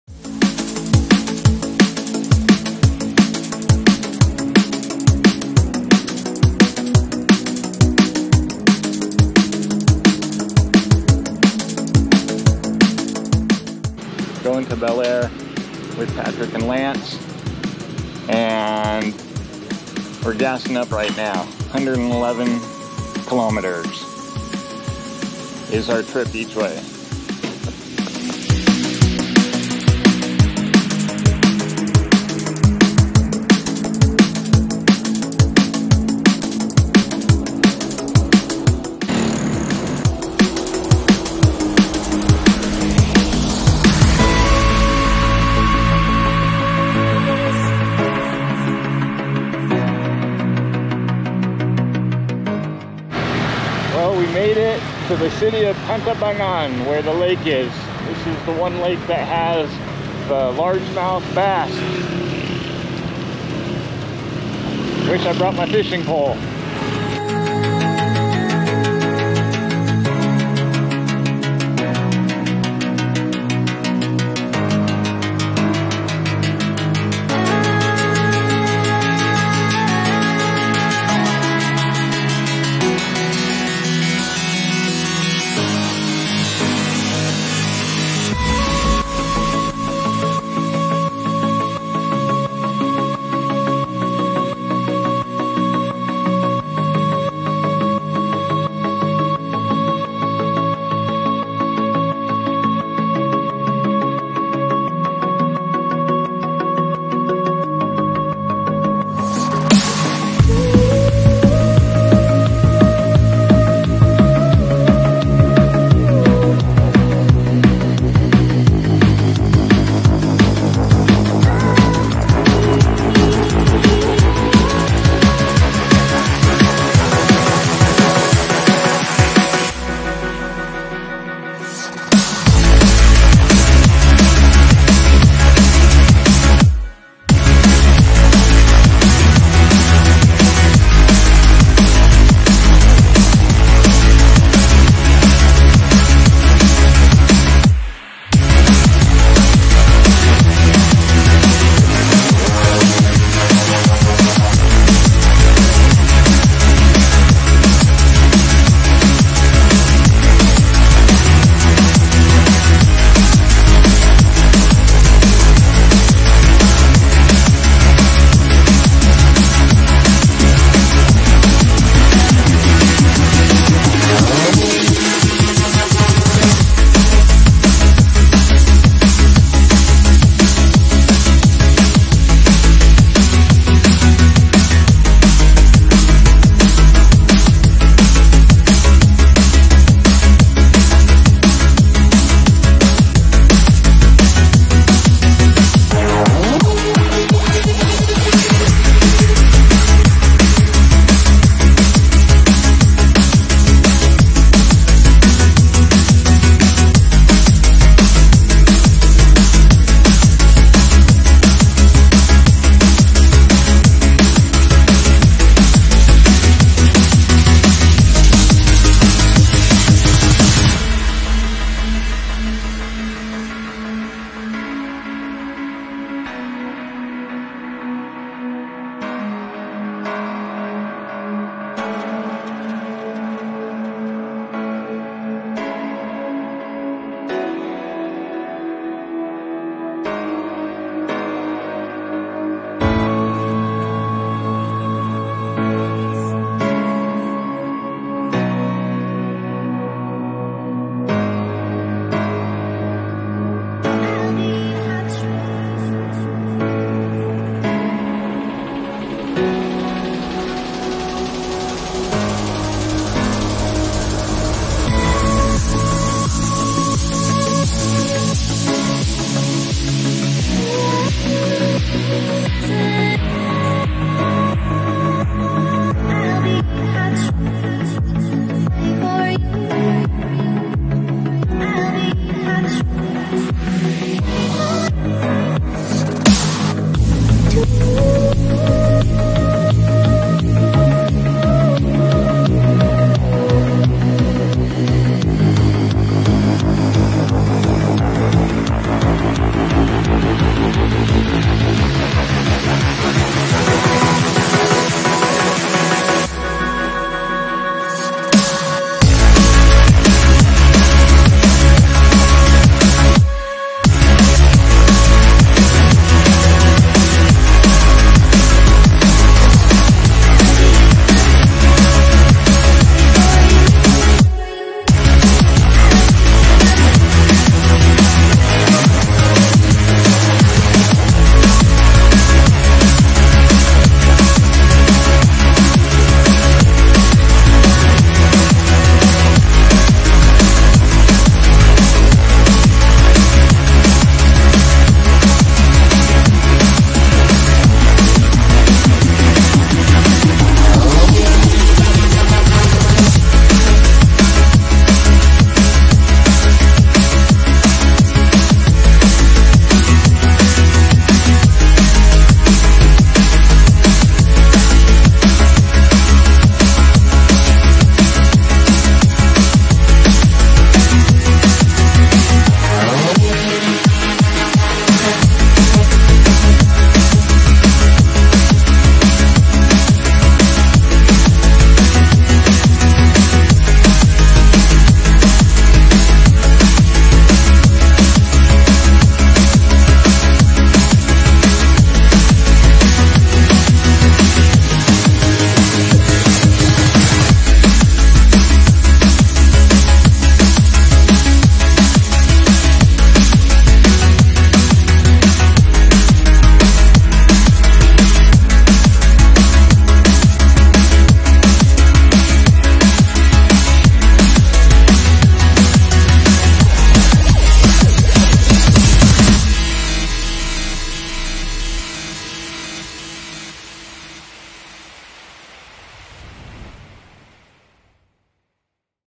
Philippines Road trip to Baler, Aurora from San Jose City, Nueva Ecija (Part 1)
While in the Philippines I went on a road trip of  111km+ on a scooter and motorcycle from San Jose City, Nueva Ecija to Baler, Aurora with stops in between including Pantabangan Dam with my friends Patrick and Llance.

Such beautiful provinces to travel through. The roads have many twists and turns as we travelled up and down mountains to get to the Pacific Ocean. We only spent one night in a beachfront hotel so I hope to return some day and enjoy it for a longer period of time.